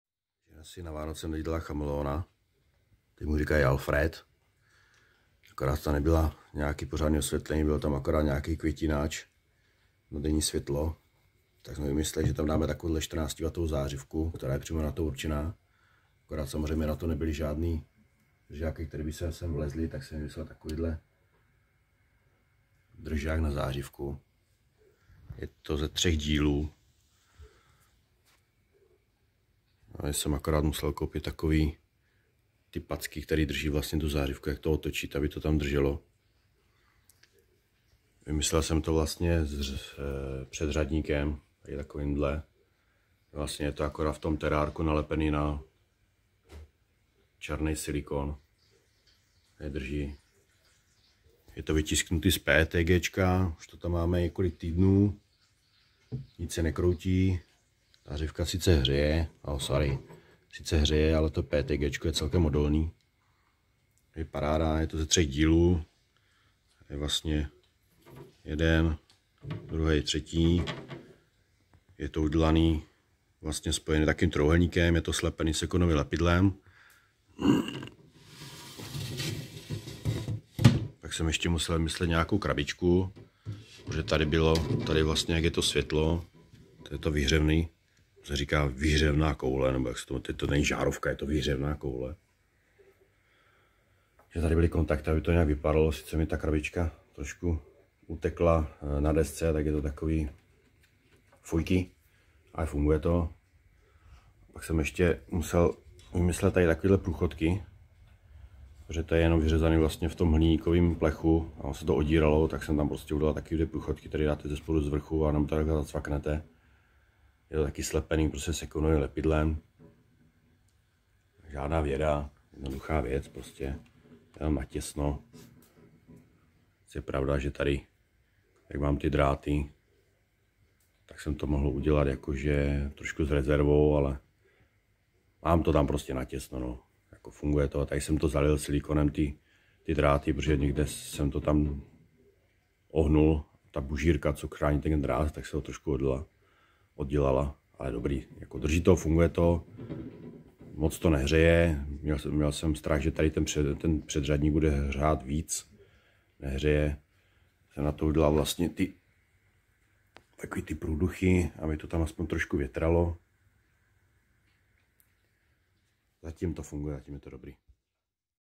Proč 3D tiskárna? - 7
Světlo pro vánoční dárek, sice pozdě, ale přece.

Tiskárna  Creality CR-6 SE
Filament PETG Aurapol